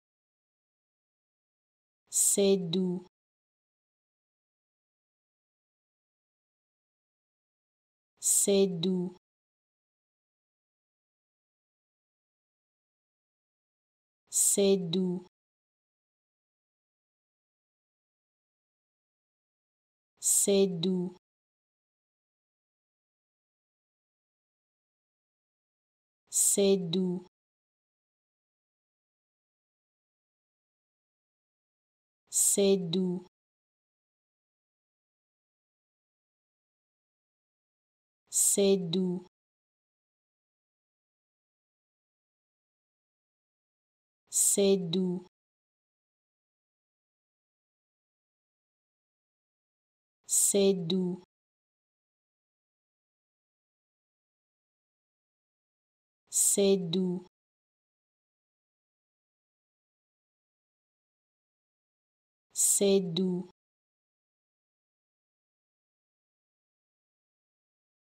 How to Pronounce ''Seydoux'' Correctly in French
Learn how to say and properly pronounce ''Seydoux'' in French with this free pronunciation tutorial. Apprenez à prononcer des mots en français grâce à nos tutoriels.